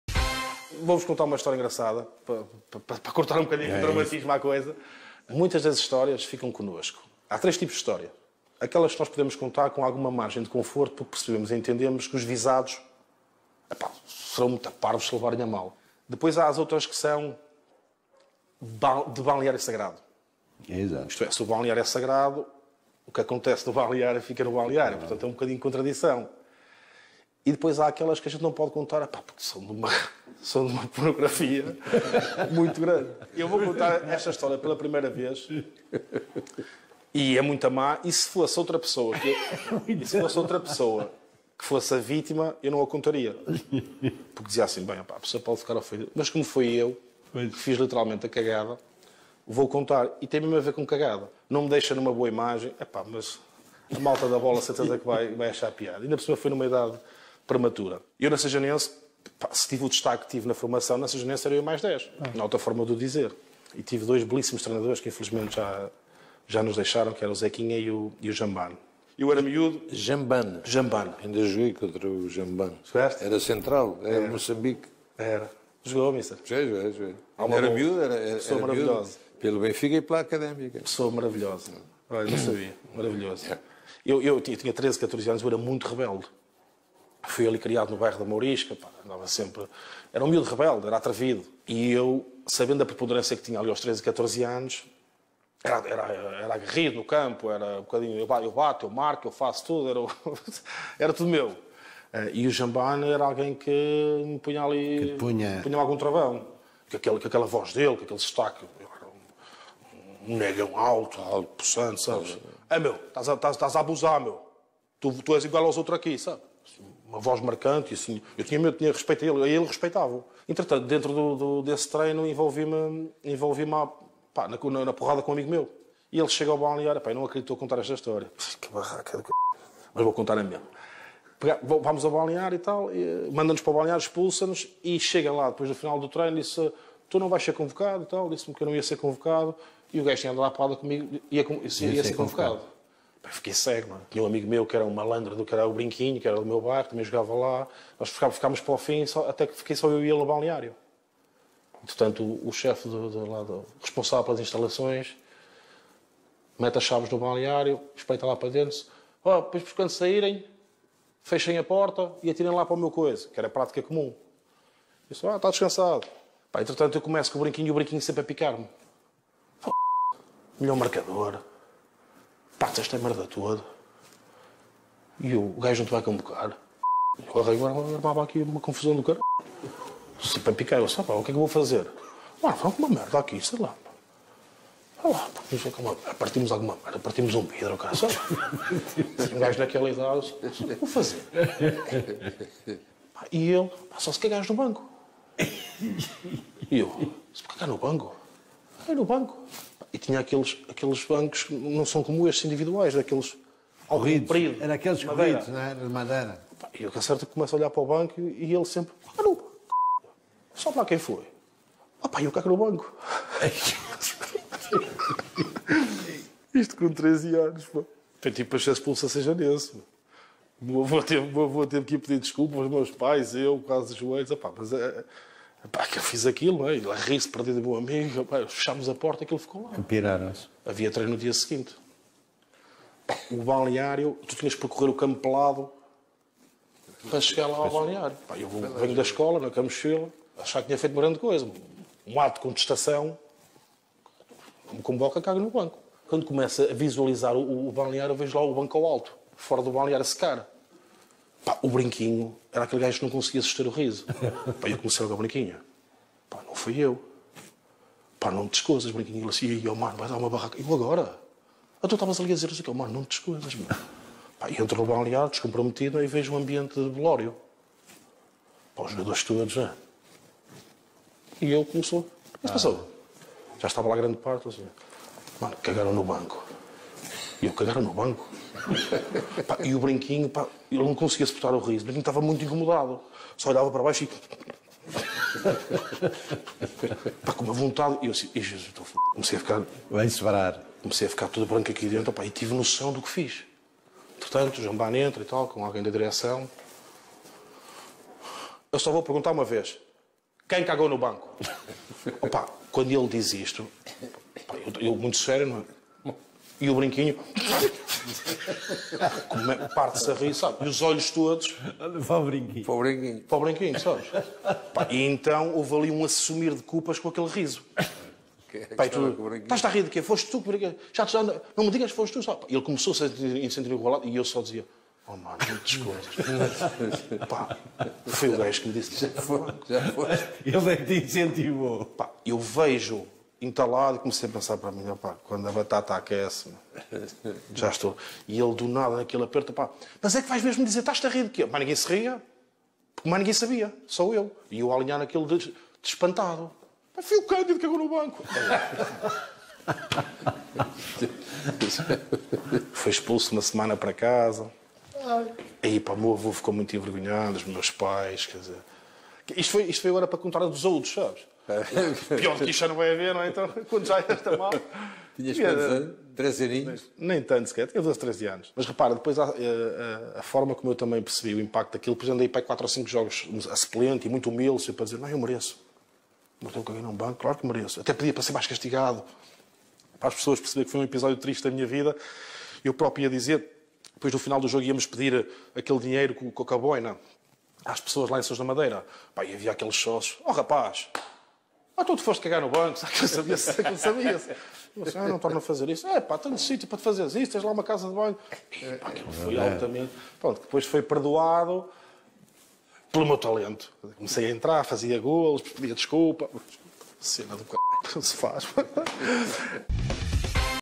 Histórias de Balneário: Cândido Costa 😂
Neste video o ex jogador Cândido Costa e agora comentador, fala sobre uma historia engraçada, de quando era pequeno e que cagou no banco